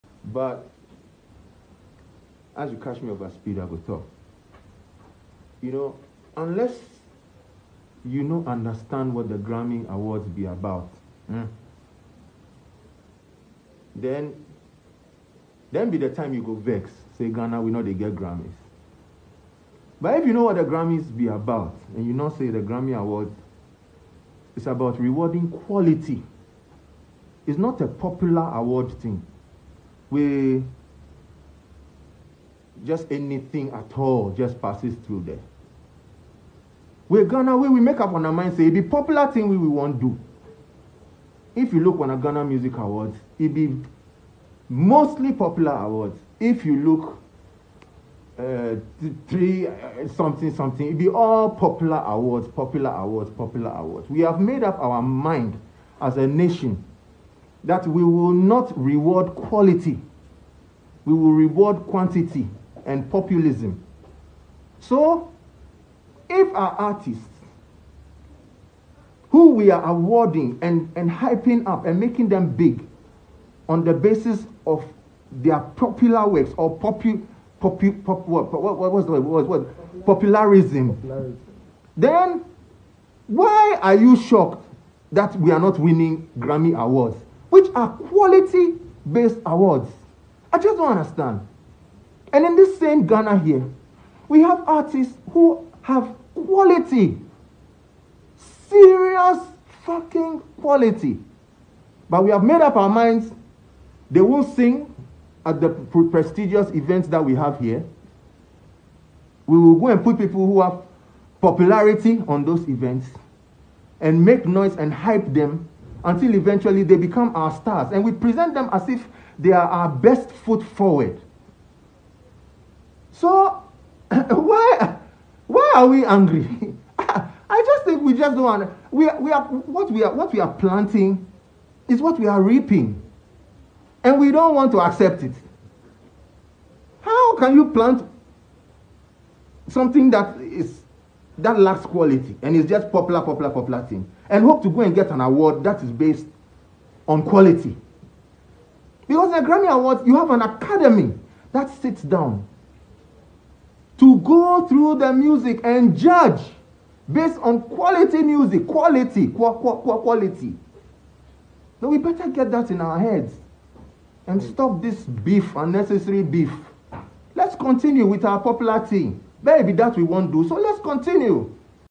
Ghana can never win a Grammy Awards because... - Dr. Knii Lante
Nii Lante Okunka Blankson, professionally known as Knii Lante, is a Ghanaian reggae, soul, and Afro pop musician, songwriter, and medical doctor from Accra, Ghana. Knii Lante is a medical doctor who has a degree in BSC Human Biology, MBCHB and MWACP. He started his music career when he was still in medical school